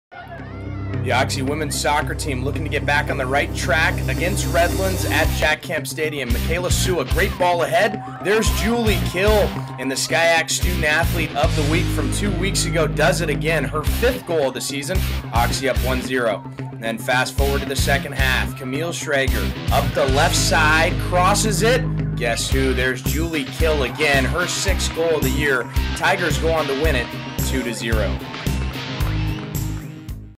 HIGHLIGHTS: Occidental 2, Redlands 0 (women's soccer)
Julie Khil scores two goals as the Tigers beat Redlands 2-0 on Oct. 8, 2014.